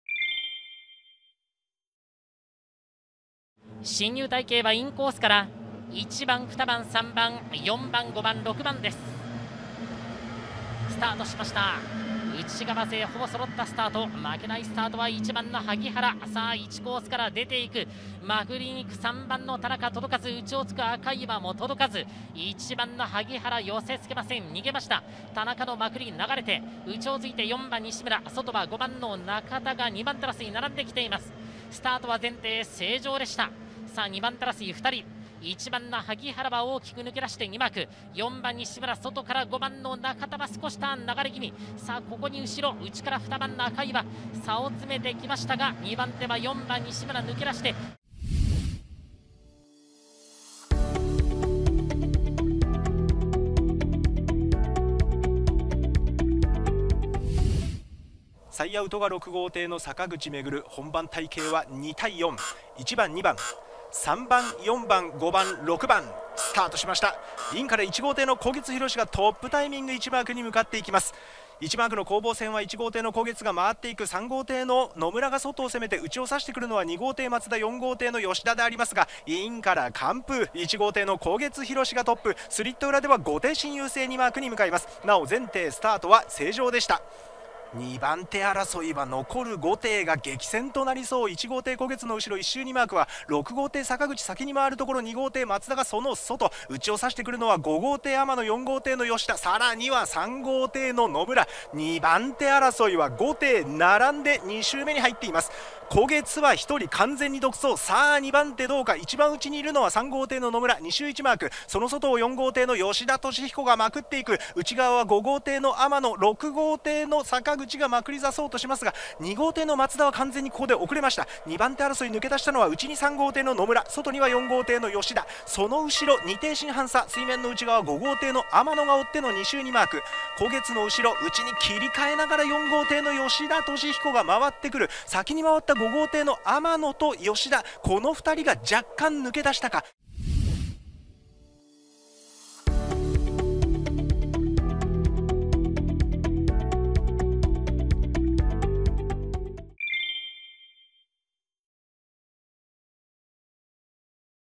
今日の徳山・常滑優勝戦を一気見せ！ 優勝したのは？│BOATCAST NEWS 2023年8月5日│
今日の徳山・常滑優勝戦を一気見せ！ 優勝したのは？
徳山一般 最終日12R・常滑一般 最終日12Rをご覧ください！